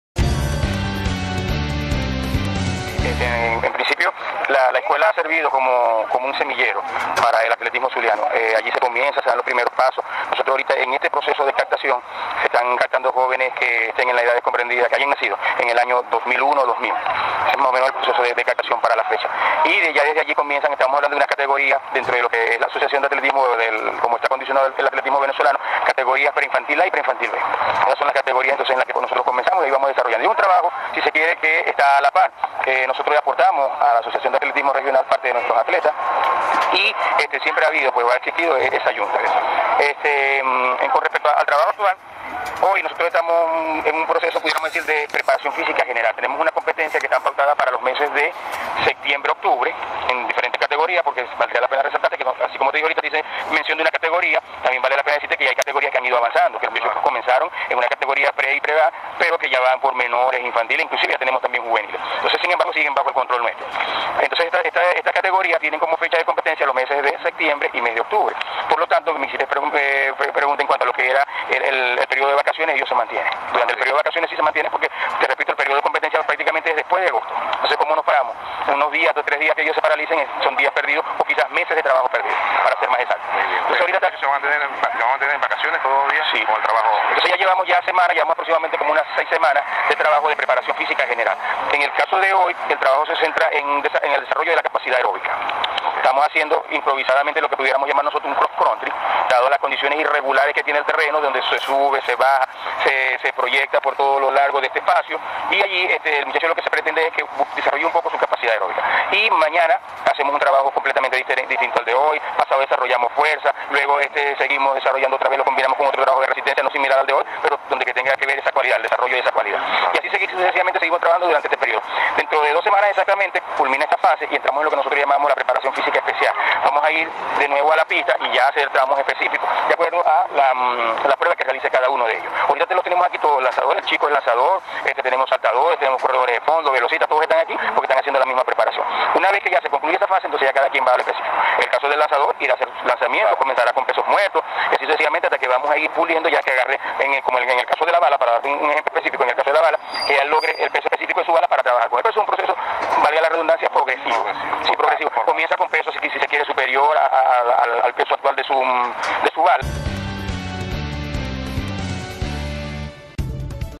Atletismo del "José Beracasa" no se va de vacaciones
Irse de vacaciones y parar los entrenamientos significa, para los atletas de la escuela de formación deportiva Don José Beracasa, perder un semestre de preparación.
Por eso, los alumnos de este liceo deportivo asentado en el polideportivo marabino, continuarán sus entrenamientos por todo el mes de agosto y tendrán que olvidarse del asueto vacacional.